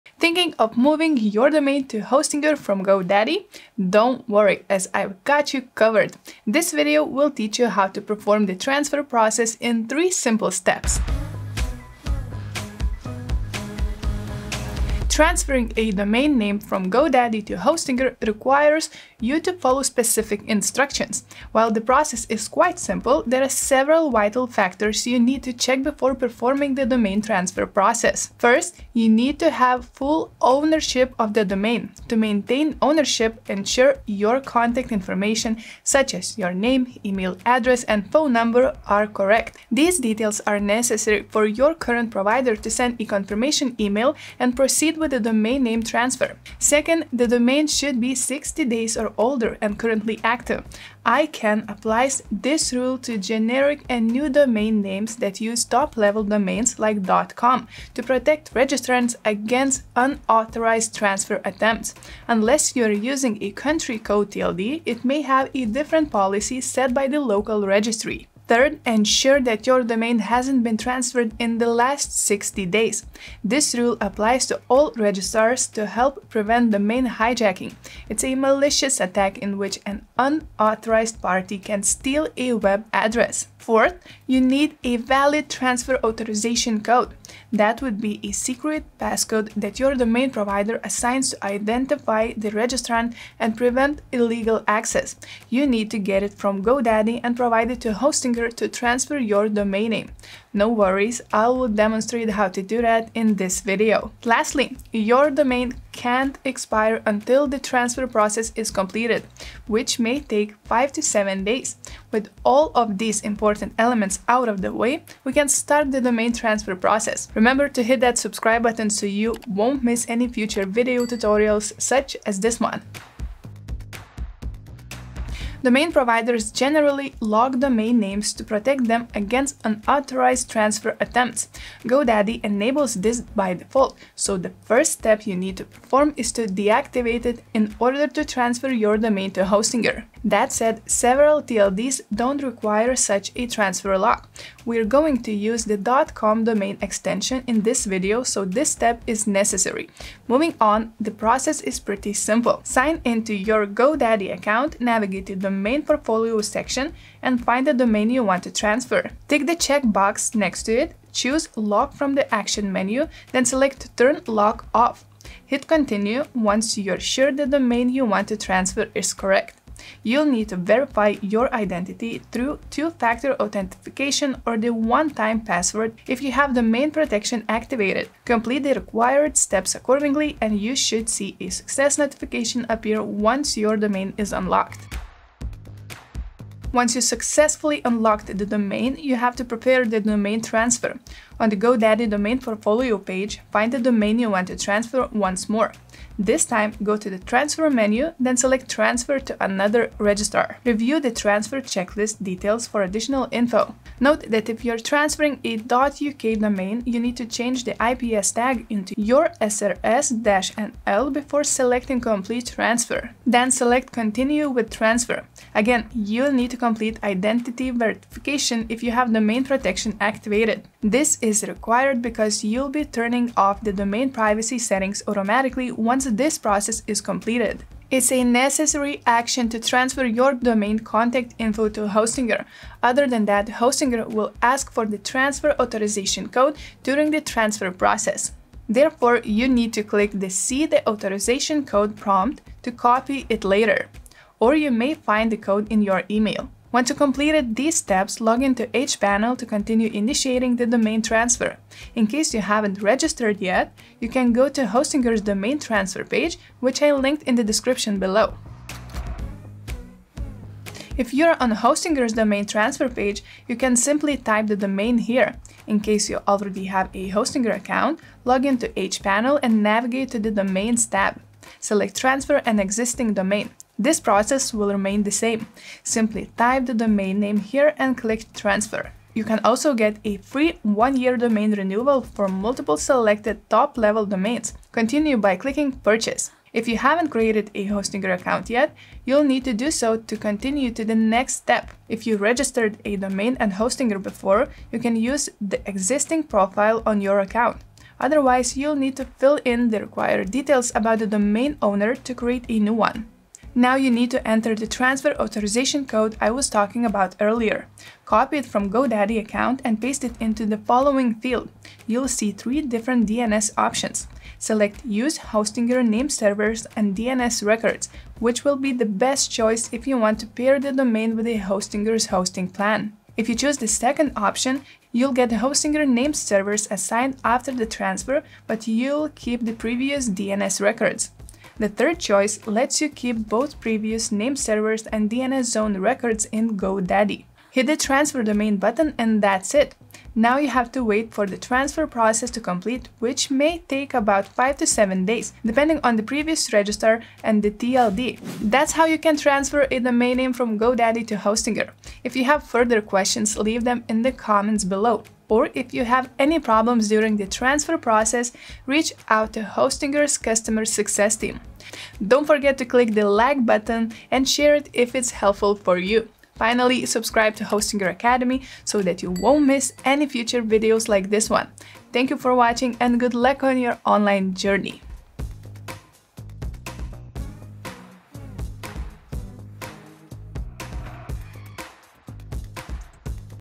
How to Transfer Domain From GoDaddy to Hostinger (2025) | Follow Along Tutorial
Learn how to transfer domain name from GoDaddy to Hostinger! Make Hostinger your website’s home

Thinking of transferring your domain to Hostinger? Follow along with the hands-on guide on how to transfer domain name from GoDaddy to Hostinger in this video.

⭐ Follow Us ⭐

🕒 Timestamps
00:00 - Introduction
02:04 - Unlock the Domain at GoDaddy
03:04 - Obtain the Authorization Code
04:19 - Initiate the Domain Transfer at Hostinger
📌 How do you transfer domain name from GoDaddy to Hostinger?
The domain transfer process is relatively simple:
1️⃣ Unlock the domain at GoDaddy
2️⃣ Obtain the authorization code
3️⃣ Initiate the domain transfer at Hostinger
However, before proceeding to transfer the domain, you need to ensure that:
✅ You have a full ownership of the domain
✅ Your domain is 60 days or older and currently active
✅ Your domain hasn’t been transferred in the last 60 days
✅ You have a valid EPP/Auth-Code
✅ Your domain won’t expire until the transfer process is completed